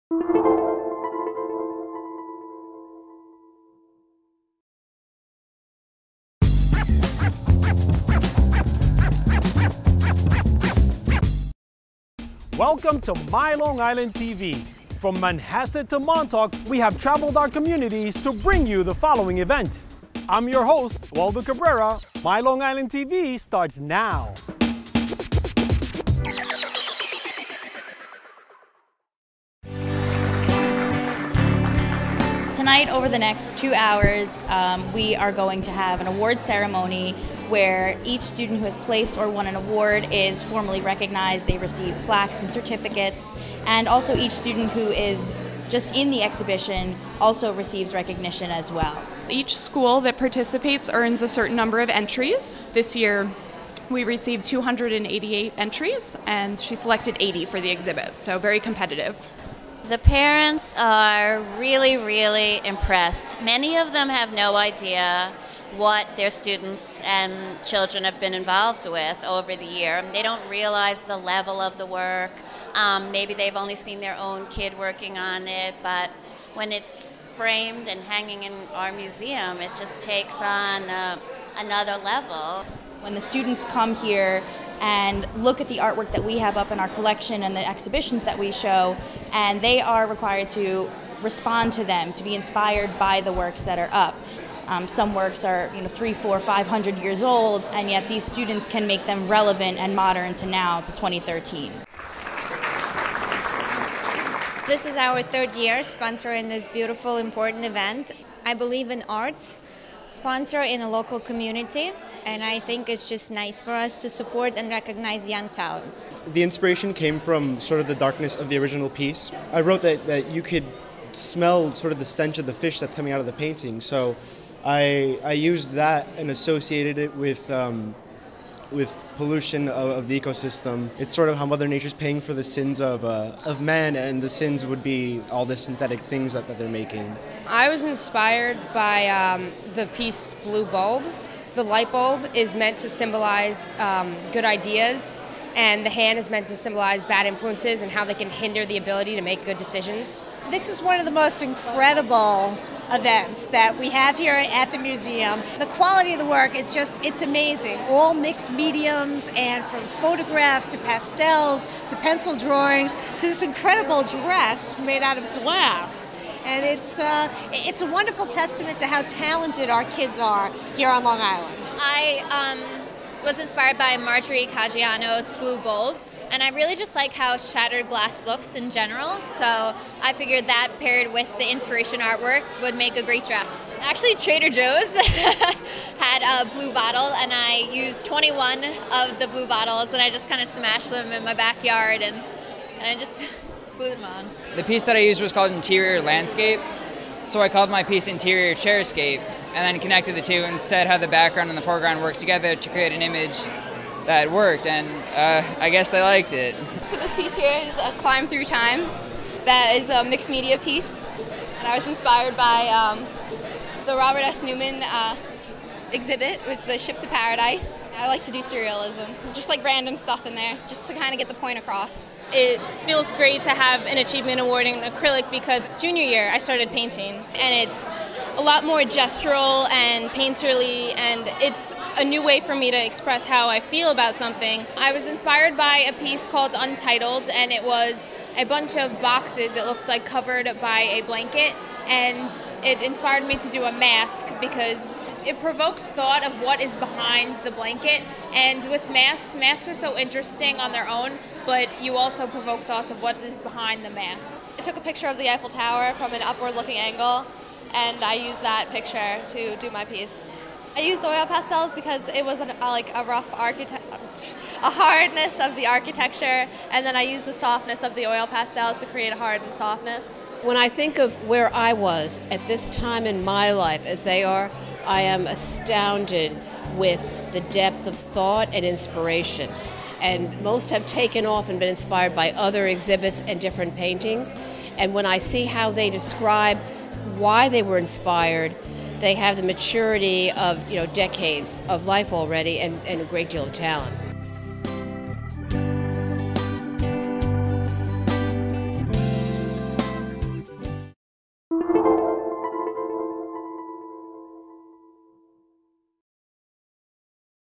The Heckscher Museum of Art honors some of Long Island's Best Young Artists
Long Island's Best Young Artists are on view at the Heckscher Museum of Art.

Featured Interviews: Sarah Salamone, Kristina Schaaf, and Joy Weiner, Heckscher Museum of Art; Dr. Inna Gellerman, Sponsor; Christopher, Best in Show, Brentwood H.S.; Jacqueline, Cold Spring Harbor H.S.; Susan Berland, Councilwoman, Town of Huntington; Tara, Our Lady of Mercy Academy; Bobby Moore, Manhasset H.S.; Kerianne, Kings Park H.S.; Luisa, Northport H.S.; Veronica, Floral Park Memorial H.S.; Kaylyn, Bayport-Blue Point H.S.; Beverly Bell, Chair, Heckscher Board of Trustees